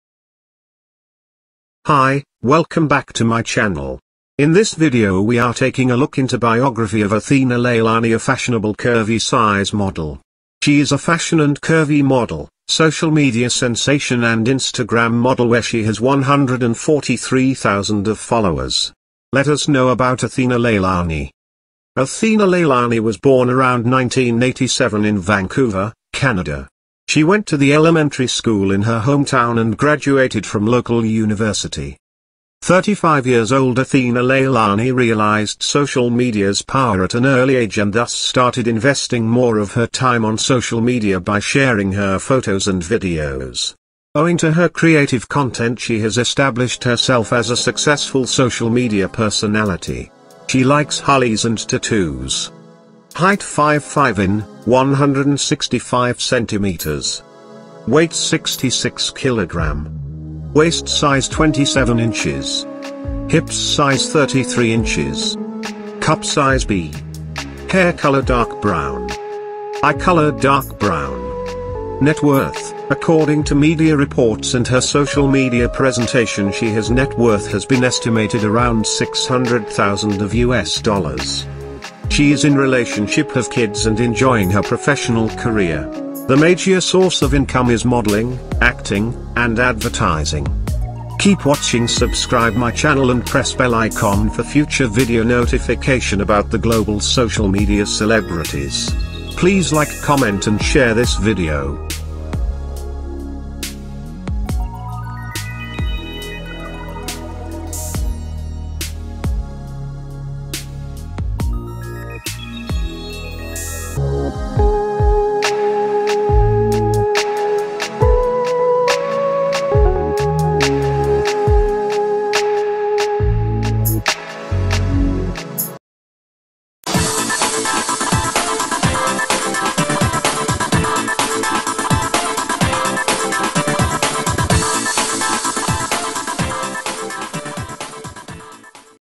Athena Leilani Wiki Biography,age,weight,relationships,net worth - Curvy models plus size
In this video we are taking a look into biography of Athena Leilani a fashionable curvy size model. She is a Fashion and Curvy model, Social media sensation and Instagram model where she has 143 thousand of followers. Let us know about Athena Leilani.